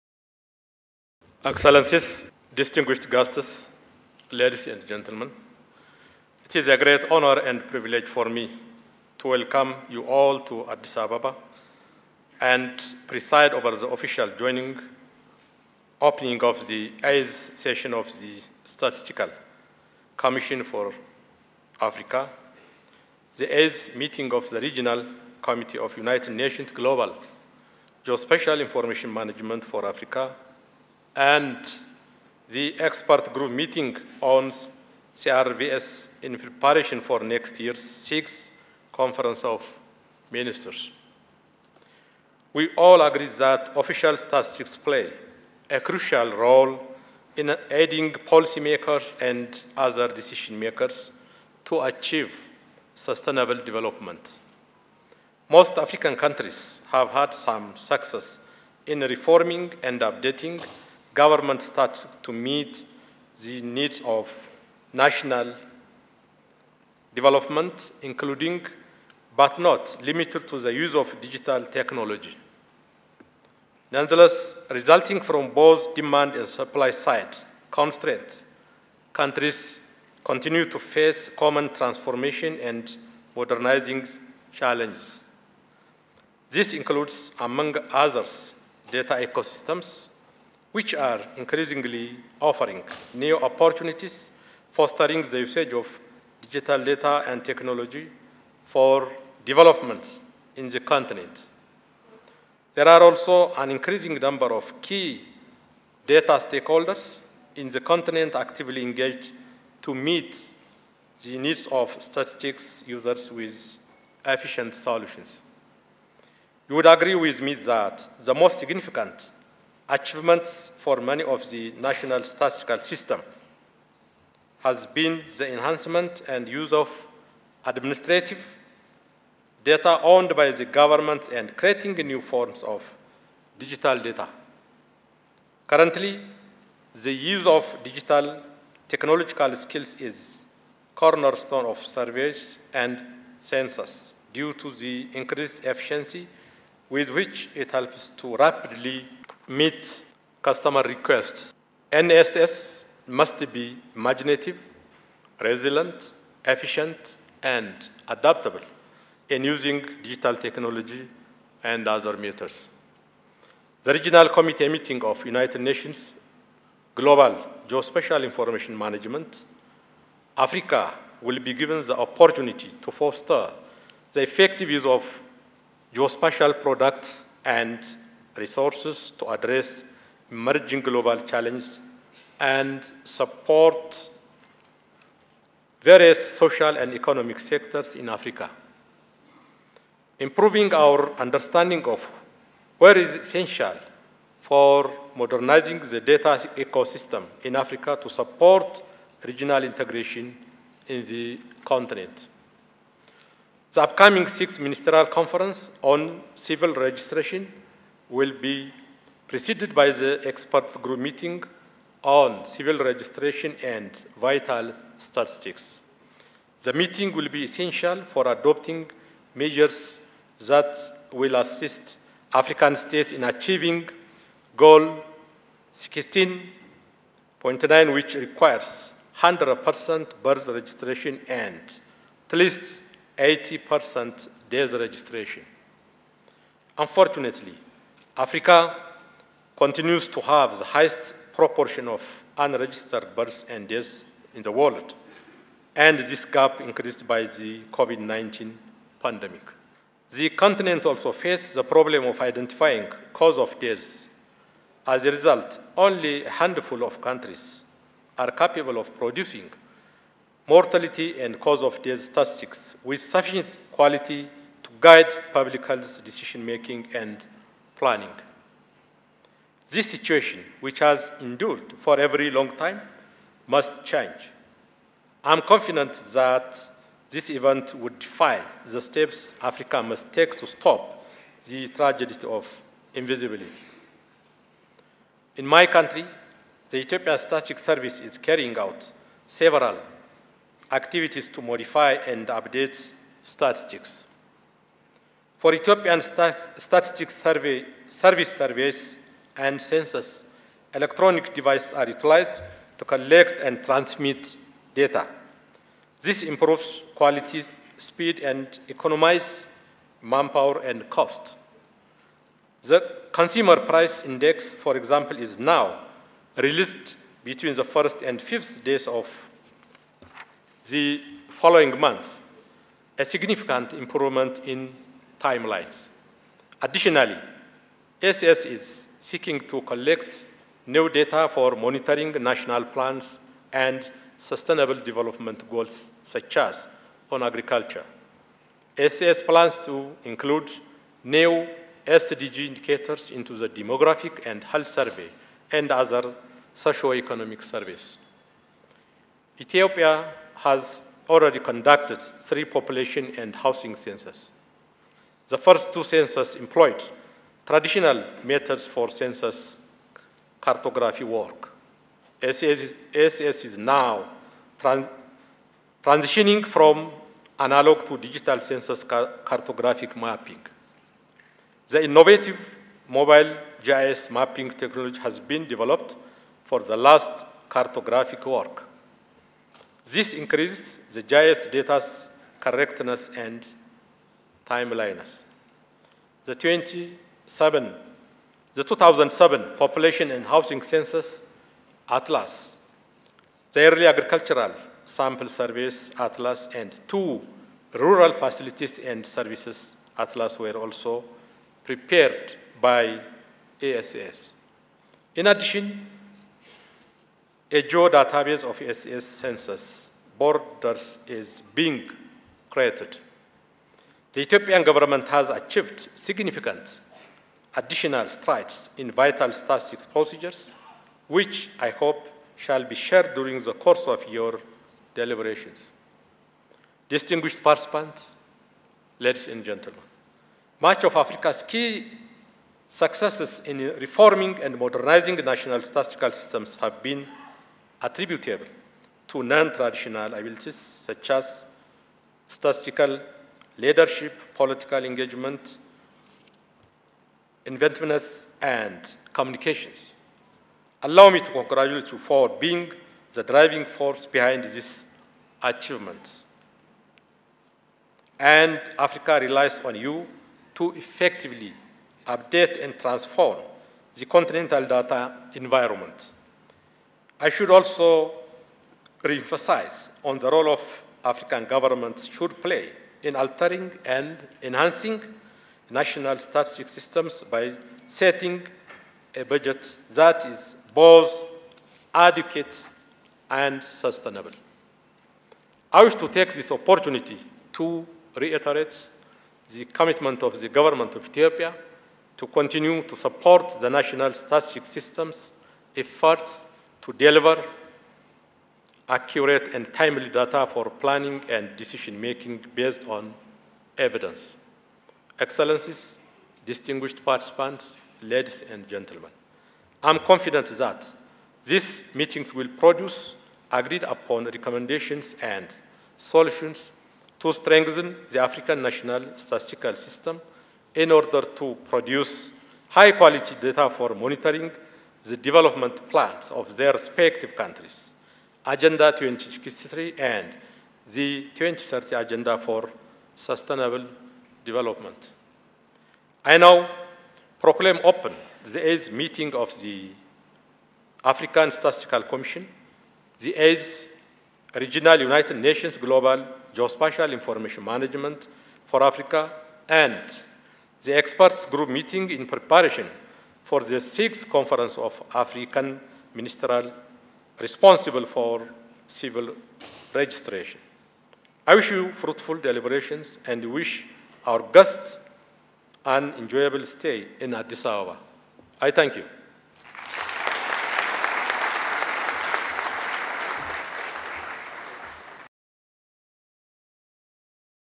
H.E Demeke Mekonnen, Deputy Prime Minister & Minister of Foreign Affairs, FDRE
Expert Group Meeting of the Conference of African Ministers Responsible for Civil Registration - 24 October 2022